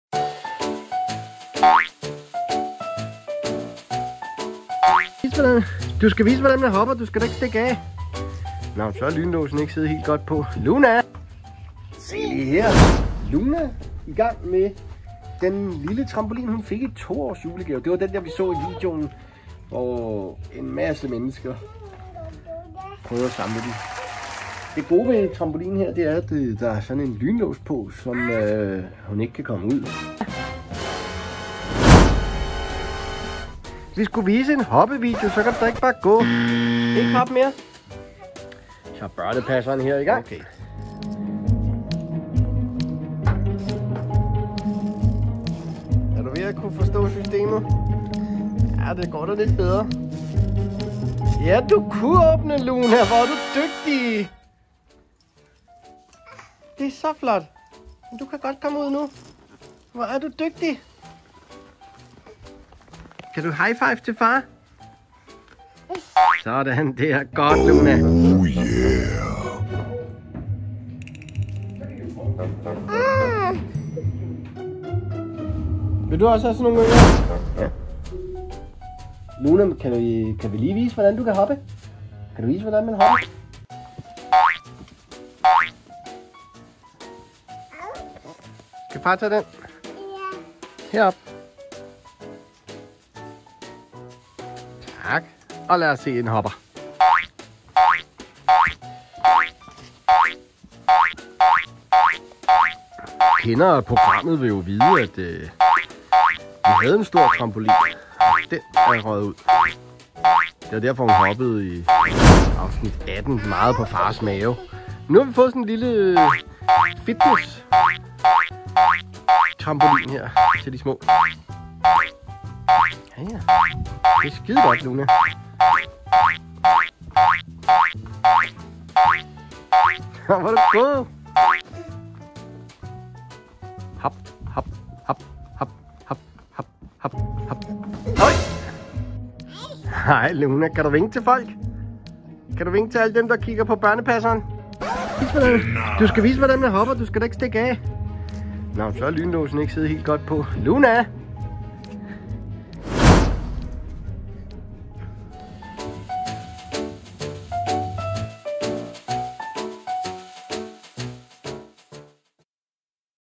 Børnepasseren #49
I disse for vort land, så alvorlige Corona tider, hvor mangt en far er hjemme med barn, eller måske bare er på weekend med barn.
Her er en række tips & tricks til gode far/barn aktiviteter. Her er episode 49, “TRAMPOLIN"
Far passer barn.